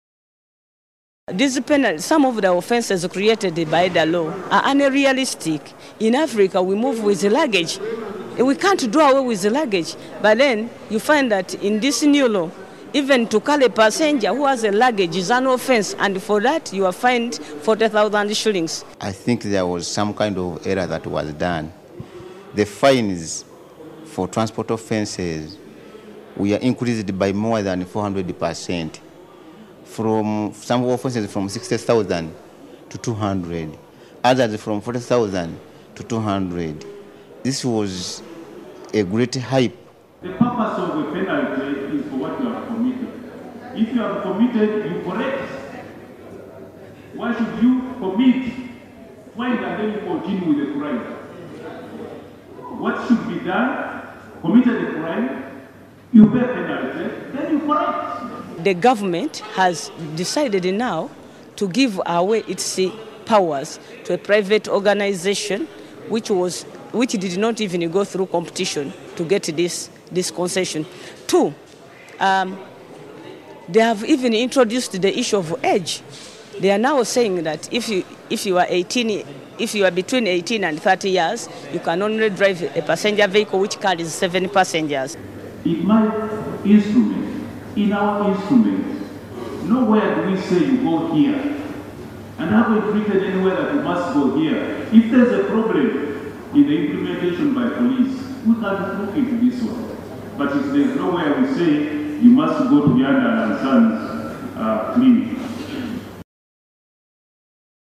PARLIAMENT ON TAXI OPERATOR STRIKE
Deputy Speaker Jacob Oulanya has asked the Works and Infrastructure committee of Parliament to look into allegations of a private driving school for public service drivers.